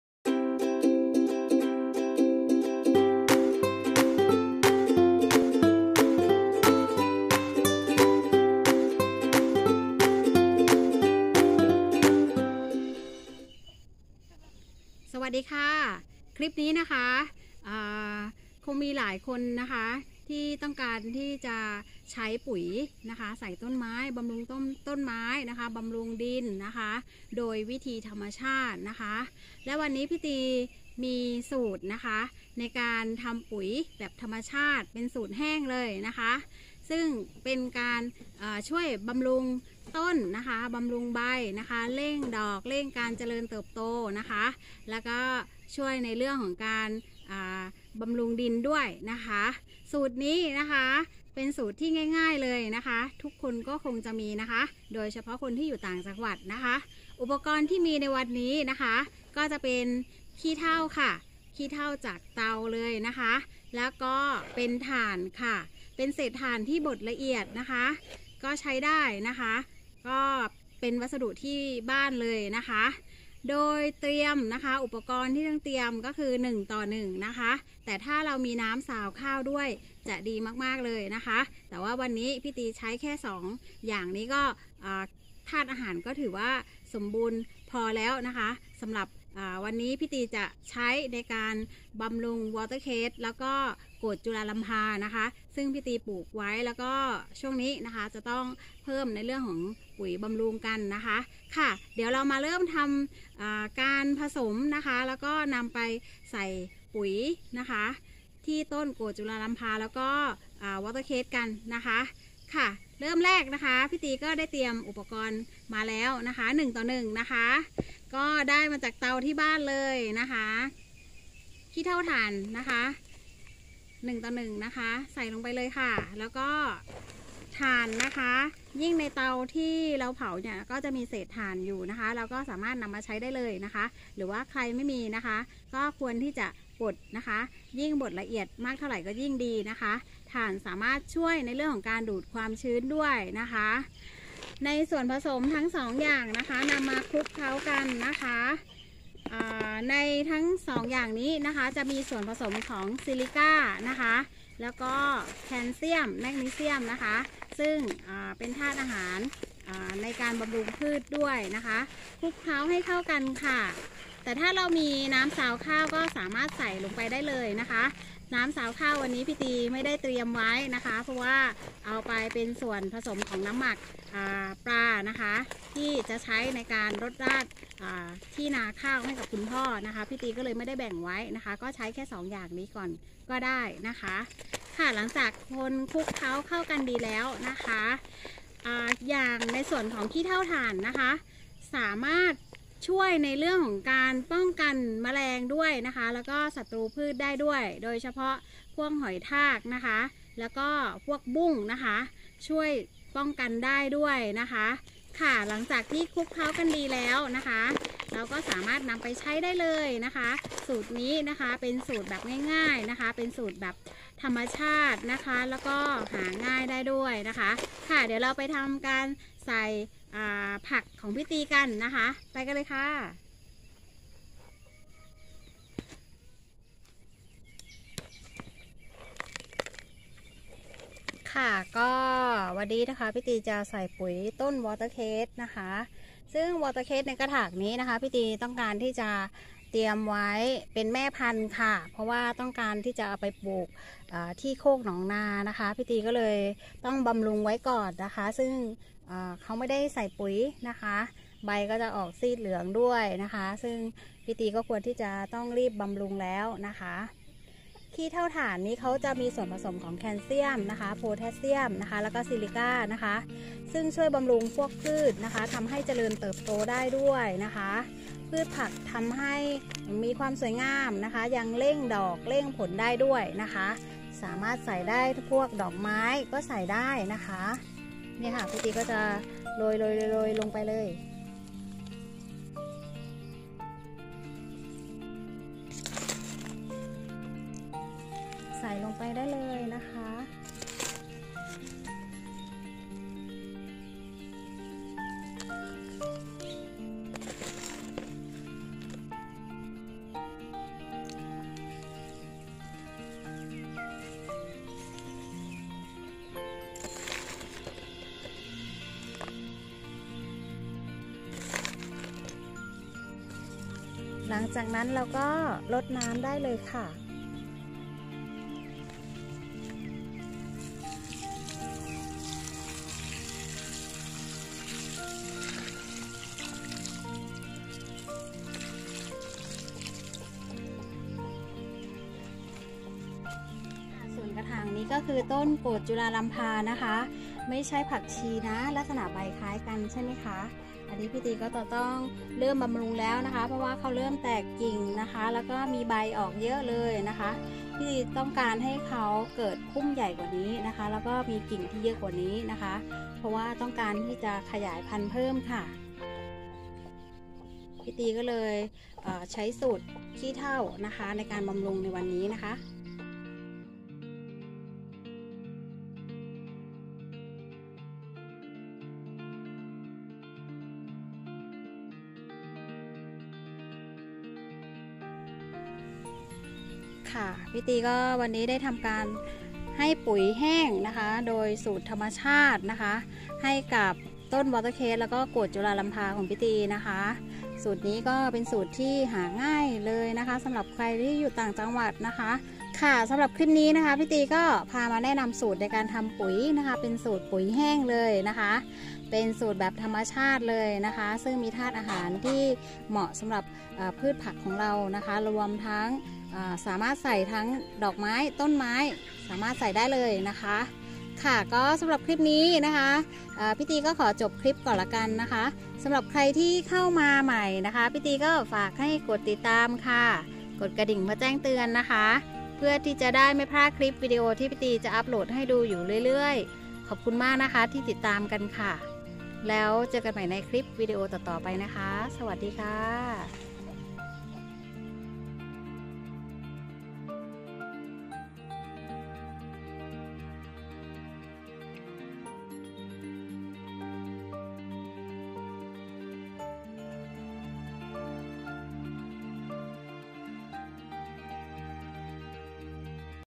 ปุ๋ยแห้งธรรมชาติสูตรบำรุงต้นใบดอก วิธีทำง่ายๆหาได้ในบ้านประหยัดงบ O บาท
คลิปนี้มีเคล็ดลับทำปุ๋ยแบบธรรมชาติกันค่ะ
หาง่ายมากแถมมีประโยชน์กับพืชผักในส่วนเราด้วย
มีแค่ 2 สิ่งนี้คือ ถ่านกับกับขี้เฒ่า แค่นี้เองค่ะ

ขอบคุณที่ติดตามกันค่ะ
Line : phanthila-73